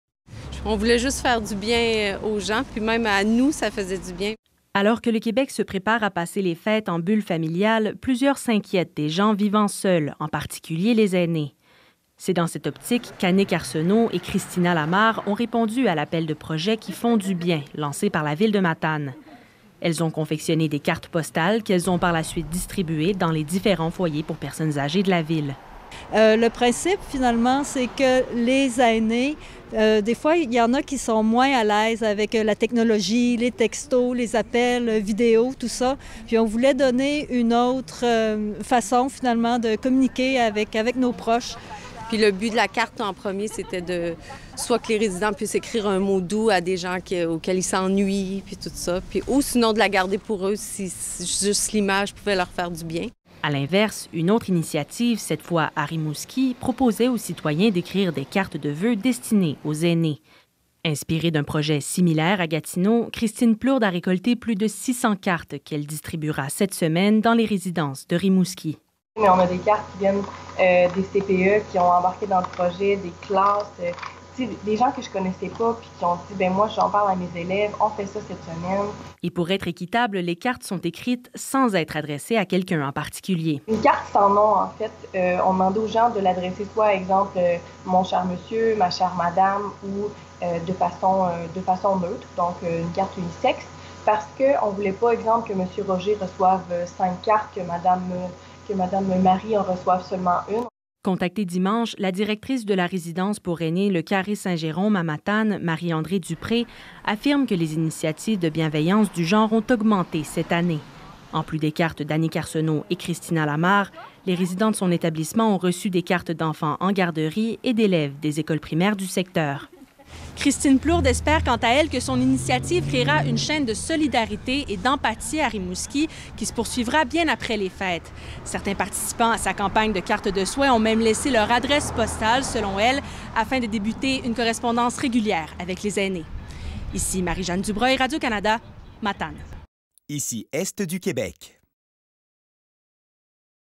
Les initiatives de réconfort se multiplient à l’approche des Fêtes
Les initiatives de bienveillance et de réconfort pour les personnes seules se multiplient dans le Bas-Saint-Laurent. À plusieurs endroits, ces initiatives ont pris la forme de cartes de souhaits pour le temps des Fêtes. Pour lire l'article complet